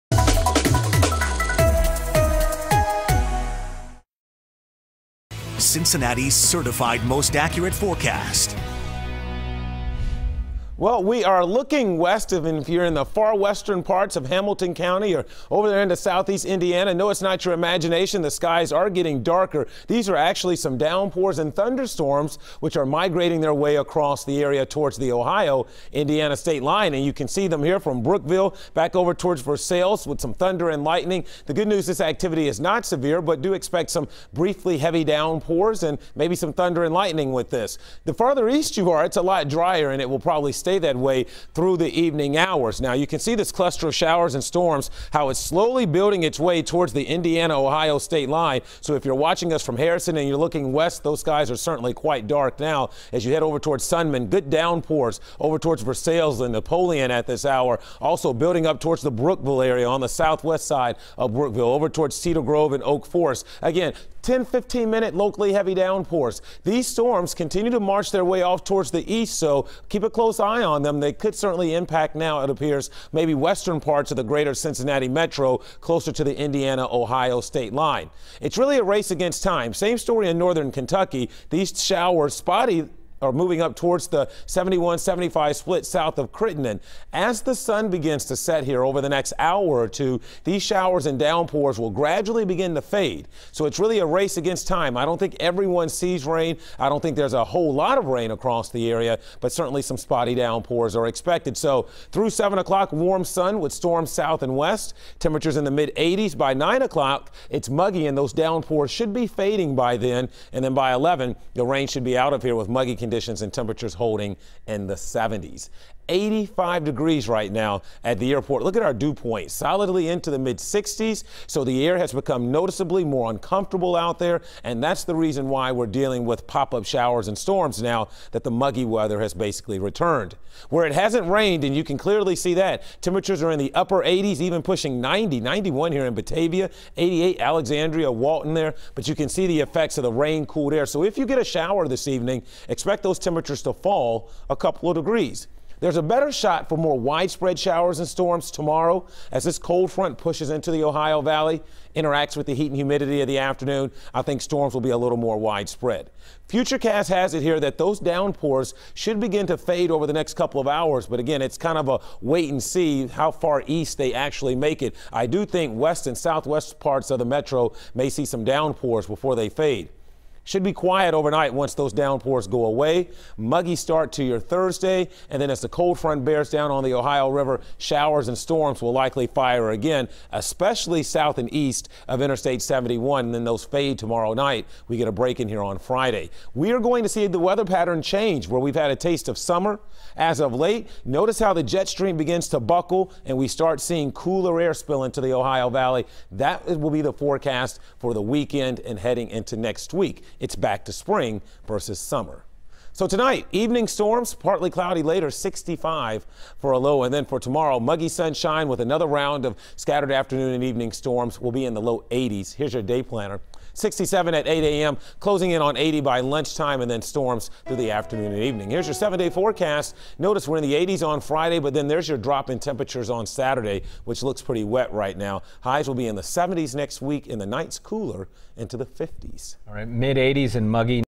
Storms Migrate East
A cool front will approach bringing a better threat for showers and storms.